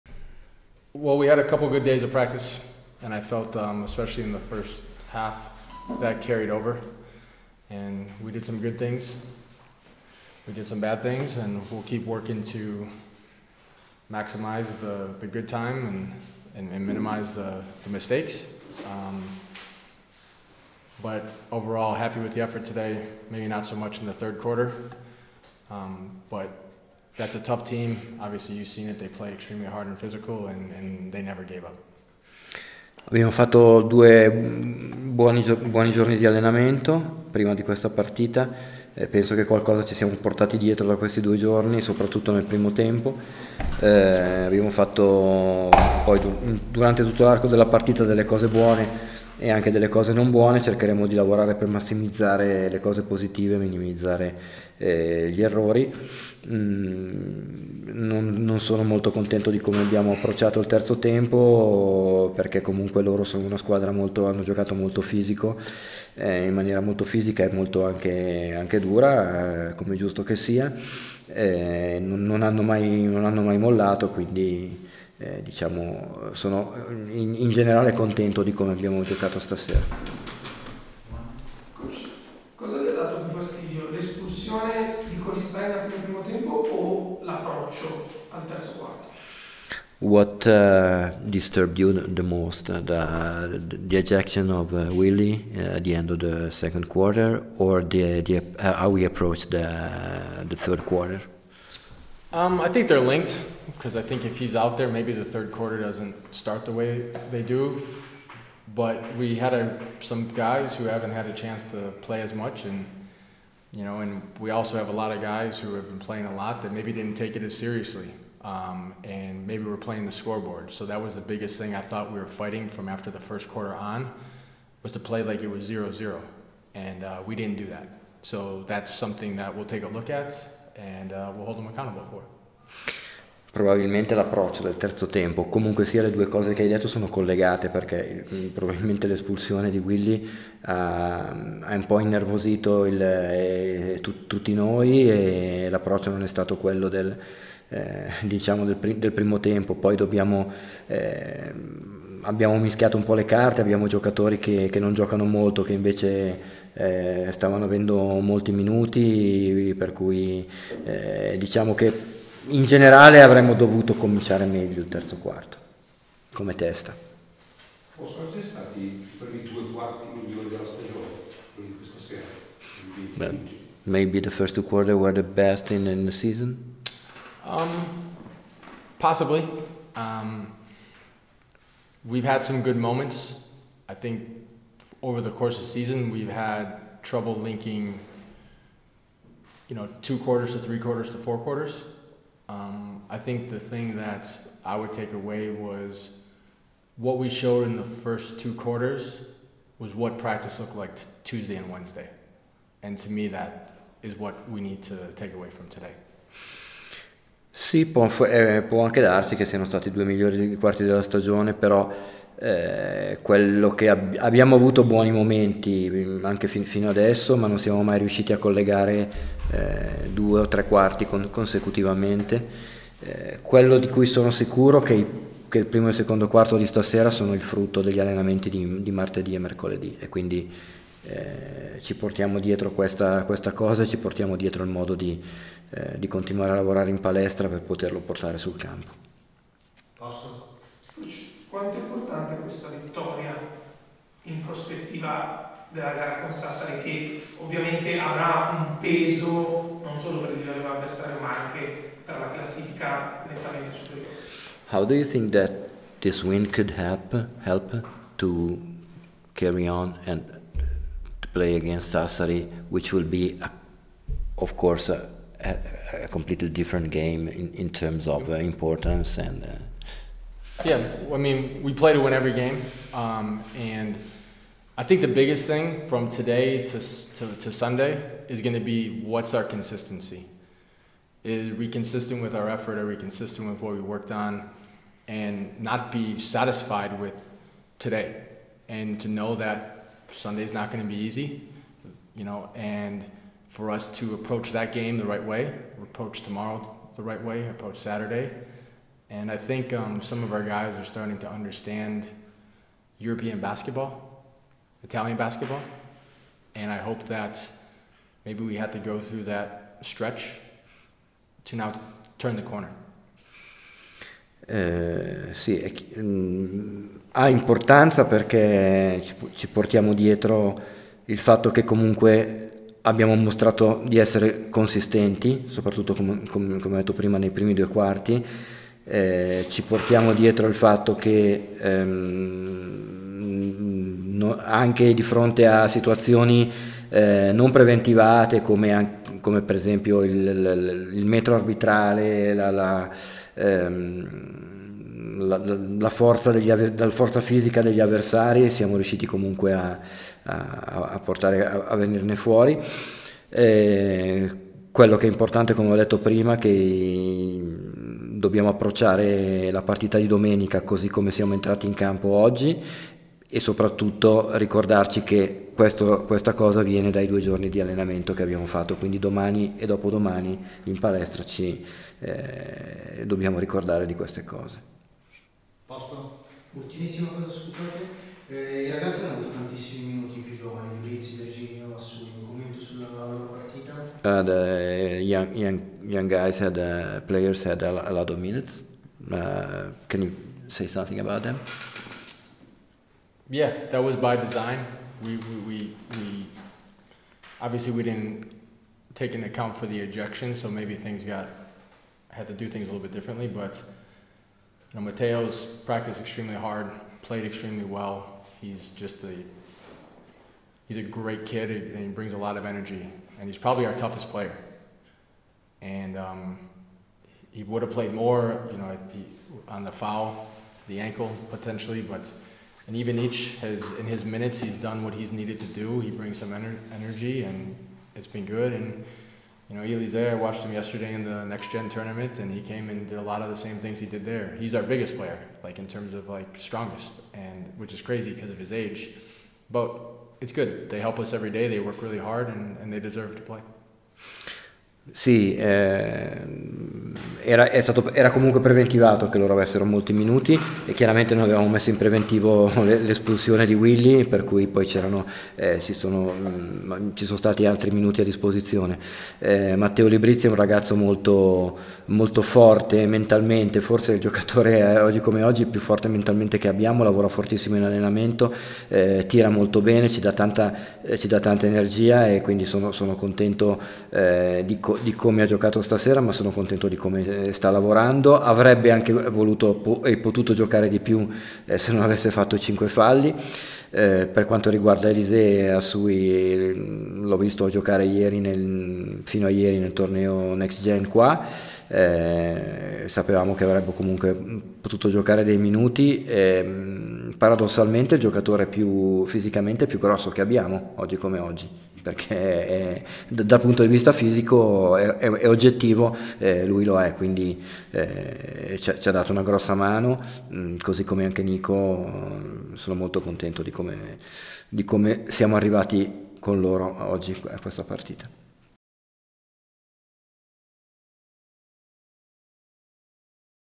Bialaszewski: «Abbiamo commesso alcuni errori, ma sono contento della vittoria»
Le parole di coach Bialaszewski al termine di Itelyum Varese-BC TSU Tbilisi